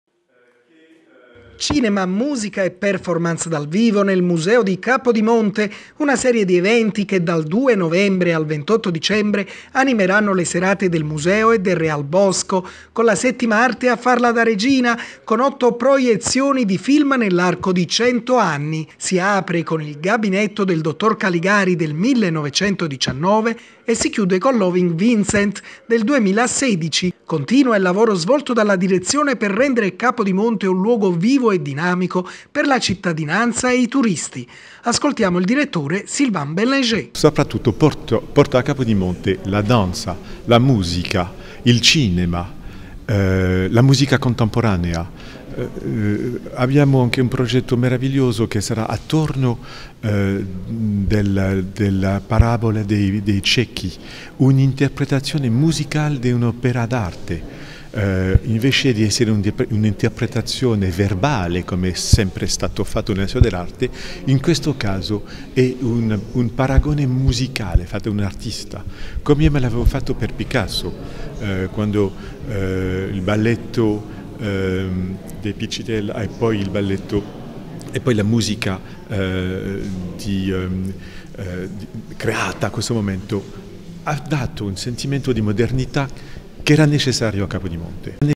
Napoli - Cinema al Museo di Capodimonte con 8 film d'autore (31.10.17)
- Napoli - Il cinema a Capodimonte con otto film d'autore nel segno dell'arte, tutti i giovedì dal 2 novembre al 29 dicembre, nell'Auditorium del Museo. E' la grande novità della rassegna 'Capodimonte dopo Vermeer' che comprende anche tre concerti nella sala della Culla e 4 performance.

"Offriamo alla città, e al quartiere in particolare, una ricca programmazione culturale di altissimo valore e per questo ringrazio vivamente la Regione Campania, che sostiene questo nostro progetto dalla forti connotazioni anche sociali", ha detto il direttore del Museo e Real Bosco di Capodimonte, Sylvain Bellenger, presentando l'iniziativa con Rosanna Romano, direttore generale per le Politiche culturali e il Turismo della Regione Campania.

La rassegna cinematografica è gratuita e si rivolge sia ai visitatori del Museo che ai cittadini, in particolare, agli abitanti di questo quartiere in cui non ci sono cinema. L'obiettivo è rendere l'Auditorium un punto di riferimento a Capodimonte, un vero e proprio cinema di quartiere". (31.10.17)